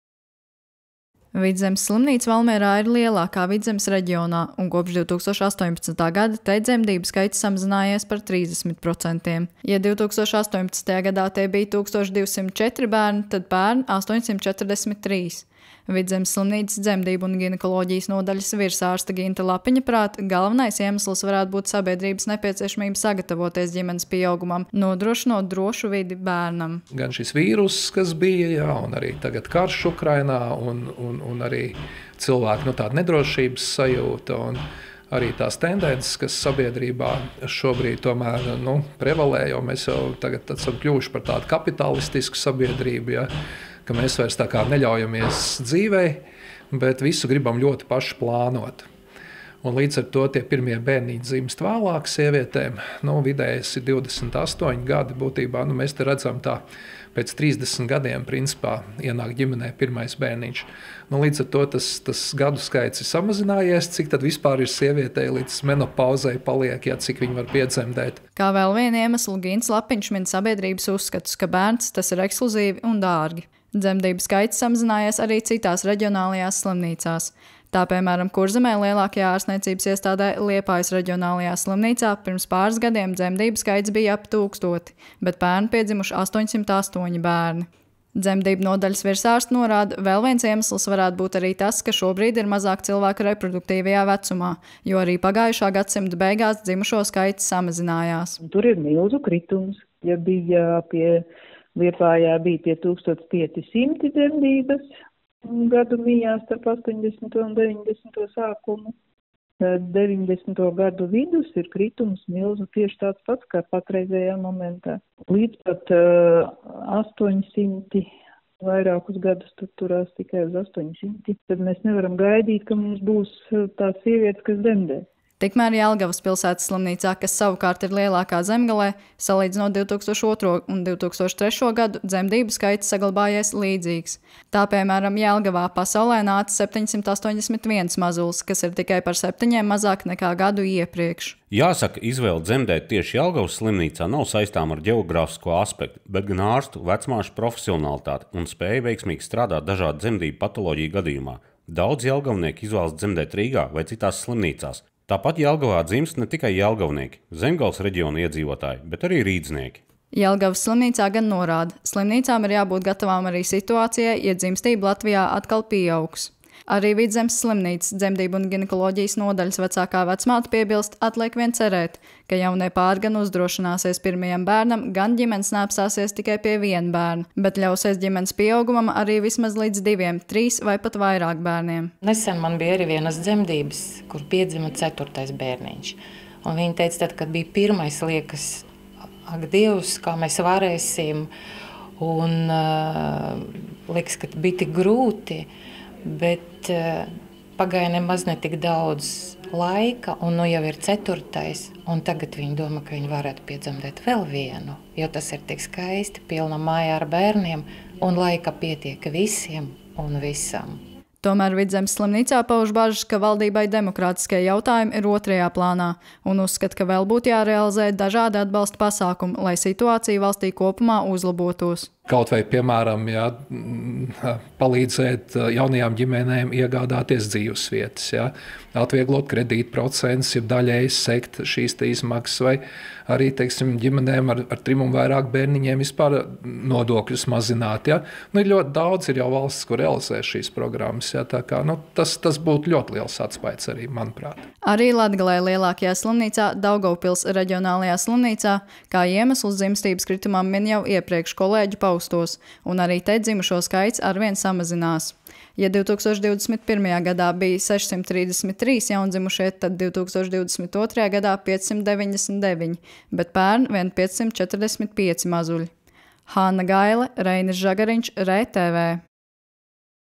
ReTV: Bērns – tas ir ekskluzīvi un dārgi
Pēdējos gadus dzimstības rādītāji Latvijā krītas arvien vairāk. Tā, piemēram, salīdzinot 2022. un 2023. gadu, dzemdību skaits ir samazinājies par 11,5%. Pēc Centrālās statistikas pārvaldes datiem, pērn piedzimuši 14 121 bērni. Uzrunājot reģionālās slimnīcas, tajās min vairākus iemeslus: šī brīža ģeopolitisko situāciju, finansiālos jautājumus un citus apstākļus.

Vidzemes slimnīca Valmierā ir lielākā Vidzemes reģionā. Kopš 2018. gada te dzemdību skaits samazinājies par 30%. Ja 2018. gadā tie bija 1204 bērni, tad pērn – 843.

Vidzemes slimnīcas Dzemdību un ginekoloģijas nodaļas virsārsts Gints Lapiņš uzskata, galvenais iemesls varētu būt sabiedrības nepieciešamība sagatavoties ģimenes pieaugumam, nodrošinot drošu vidi bērnam. “Gan vīruss, kas bija, tagad karš Ukrainā, un cilvēkiem nedrošības sajūta. Arī tendences, kas šobrīd ir sabiedrībā, jo mēs jau tagad kļūstam par tādu kapitālistisku sabiedrību, mēs vairs neļaujamies dzīvei, bet visu gribam ļoti paši plānot. Pirmie bērniņi dzimst sievietēm vēlāk. Vidējais ir 28 gadi. Pēc 30 gadiem ienāk ģimenei pirmais bērniņš. Tas gadu skaits ir samazinājies, cik tad vispār sievietei līdz menopauzei paliek, cik viņa var piedzemdēt.”

Kā vēl vienu iemeslu Gints Lapiņš min sabiedrības uzskatus, ka bērns – tas ir ekskluzīvi un dārgi. Dzemdību skaits samazinājies arī citās reģionalājās slimnīcās. Tā, piemēram, Kurzemē lielākajā ārstniecības iestādē – Liepājas reģionālajā slimnīcā – pirms pāris gadiem dzemdību skaits bijis ap tūkstoti, bet pērn piedzimuši 808 bērni. Dzemdību nodaļas virsārste Inga Vēvere norāda, vēl viens iemesls varētu būt arī tas, ka šobrīd ir mazāk cilvēku reproduktīvajā vecumā, jo arī pagājušā gadsimta beigās dzimušo skaits samazinājās.

Tikmēr Jelgavas pilsētas slimnīcā, kas ir lielākā Zemgalē, salīdzinot 2022. un 2023. gadu, dzemdību skaits saglabājies līdzīgs. Pērn Jelgavā pasaulē nāca 781 mazulis, kas ir tikai par septiņiem mazāk, nekā bija gadu iepriekš. Jelgavas pilsētas slimnīcas valdes priekšsēdētājs Kārlis Smilga: “Jāsaka, izvēle dzemdēt tieši Jelgavas slimnīcā nav saistāma ar ģeogrāfisko aspektu, bet gan ārstu, vecmāšu profesionalitāti un spēju veiksmīgi strādāt dažādu dzemdību patoloģiju gadījumā, daudzi jelgavnieki izvēlas dzemdēt Rīgā vai citās slimnīcās, tāpat Jelgavā dzimst ne tikai jelgavnieki, Zemgales reģiona iedzīvotāji, bet arī rīdzinieki.”

Jelgavas pilsētas slimnīcā gan norāda, slimnīcām ir jābūt gatavām arī situācijai, ja dzimstība Latvijā atkal pieaugs.

Arī Vidzemes slimnīcas Dzemdību un ginekoloģijas nodaļas vecākā vecmāte Natālija Šaramigina piebilst, atliek vien cerēt, ka jaunie pāri gan uzdrošināsies pirmajam bērnam, gan ģimenes neapstāsies tikai pie viena bērna, bet ļausies ģimenes pieaugumam arī līdz vismaz diviem, trīs vai pat vairāk bērniem. “Nesen man bija arī vienas dzemdības, kur piedzima ceturtais bērniņš. Viņa teica, kad bija pirmais, liekas – “ak, Dievs, kā mēs varēsim?” Liekas, ka bija tik grūti, bet pagāja nemaz ne tik daudz laika, nu jau ir ceturtais. Tagad viņa domā, ka viņa varētu piedzemdēt vēl vienu, jo tas ir tik skaisti – pilna māja ar bērniem, laika pietiek visiem un visam.”

Tomēr Vidzemes slimnīcā pauž bažas, ka valdībai demogrāfiskie jautājumi ir otrajā plānā, uzskata, ka vēl būtu jārealizē dažādi atbalsta pasākumi, lai situācija valstī kopumā uzlabotos. Vidzemes slimnīcas Dzemdību un ginekoloģijas nodaļas virsārsts Gints Lapiņš: “Palīdzēt jaunajām ģimenēm iegādāties dzīves vietas, atvieglot kredītprocentus jeb daļēji segt maksas vai arī ģimenēm ar trim un vairāk bērniņiem vispār nodokļus mazināt. Ļoti daudz jau ir valstu, kas realizē šīs programmas. Manuprāt, tas būtu ļoti liels atspaids.”

Arī Latgalē lielakajā slimnīcā Daugavpils reģionālajā slimnīcā kā iemeslus dzimstības kritumam min jau iepriekš kolēģu paustos, arī te dzimušo skaits ar vien samazinās. Ja 2021. gadā bija 633 jaundzimušie, tad 2022. gadā 599, bet pērn vien 545 mazuļi.

Foto: Freepik; ilustratīvs